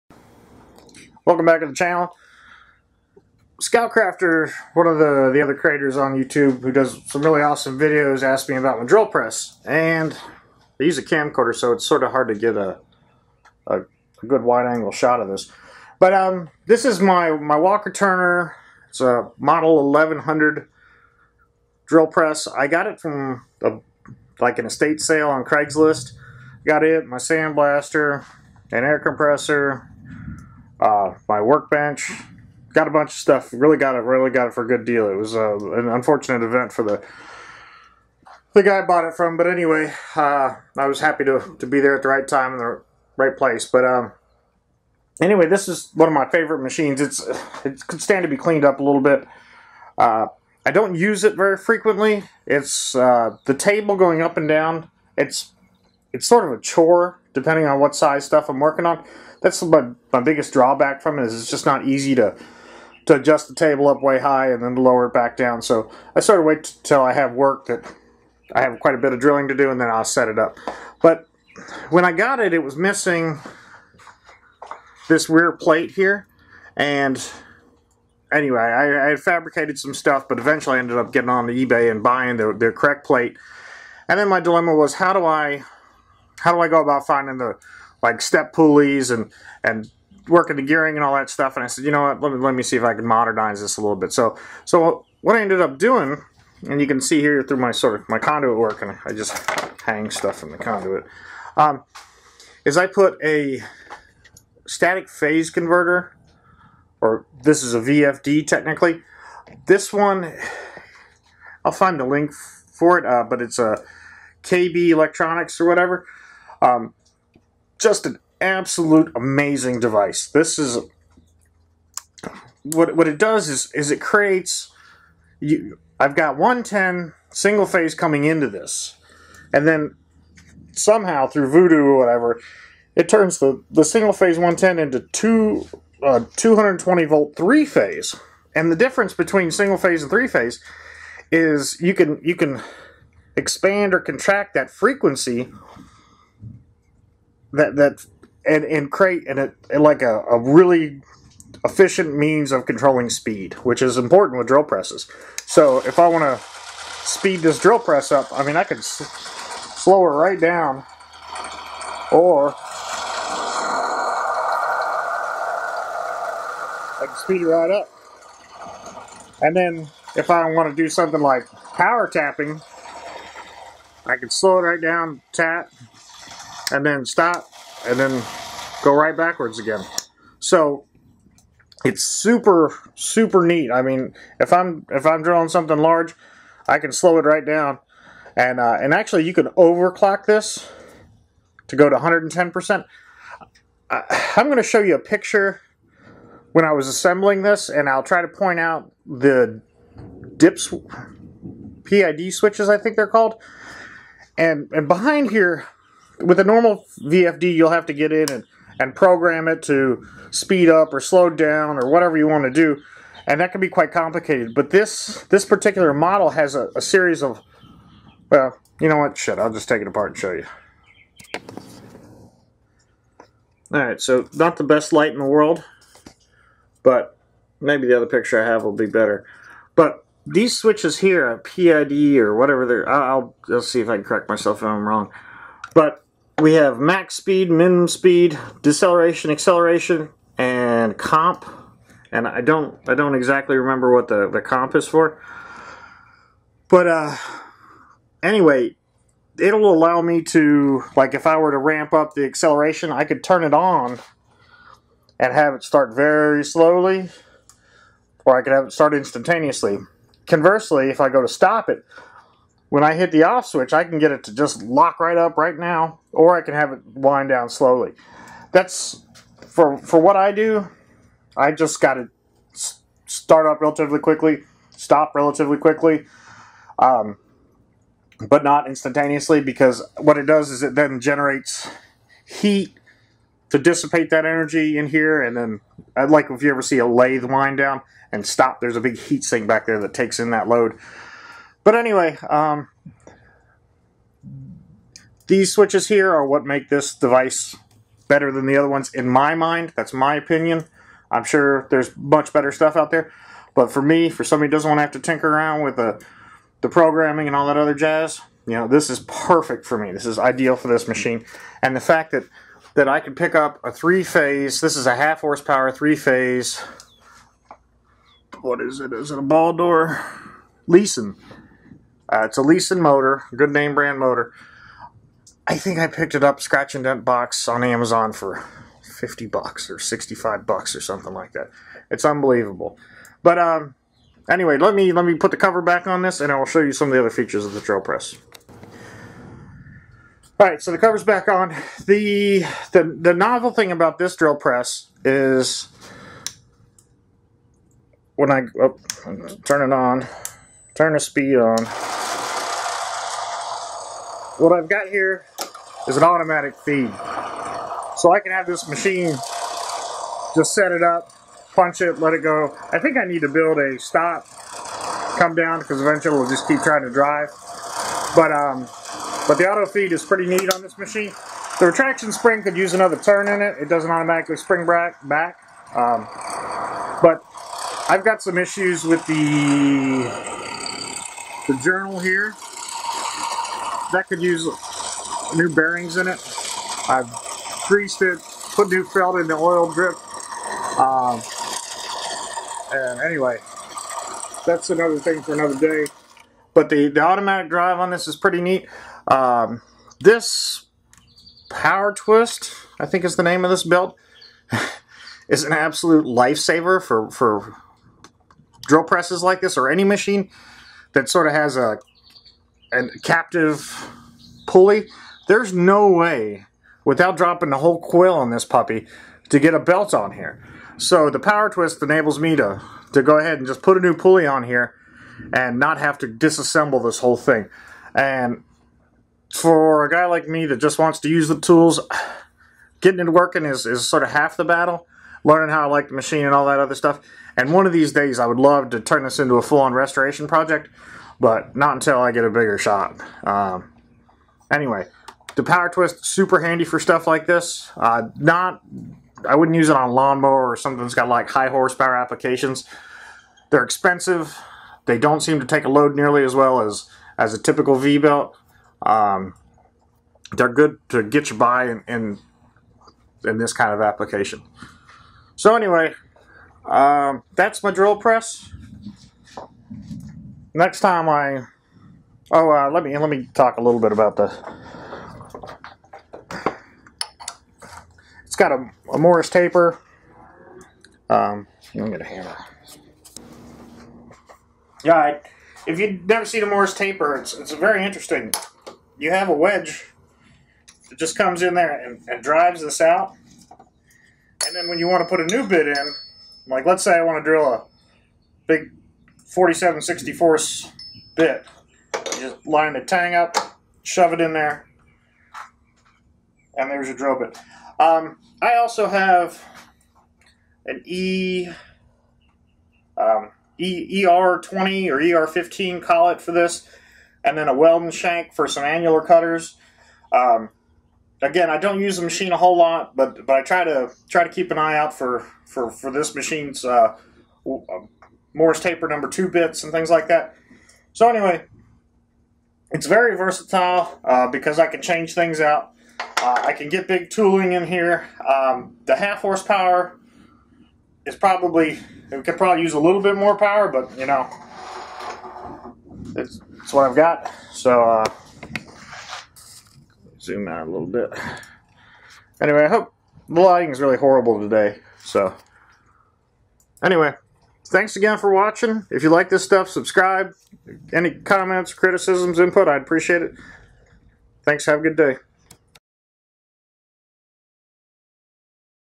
Walker Turner Drill press with VFD
A quick overview of my drill press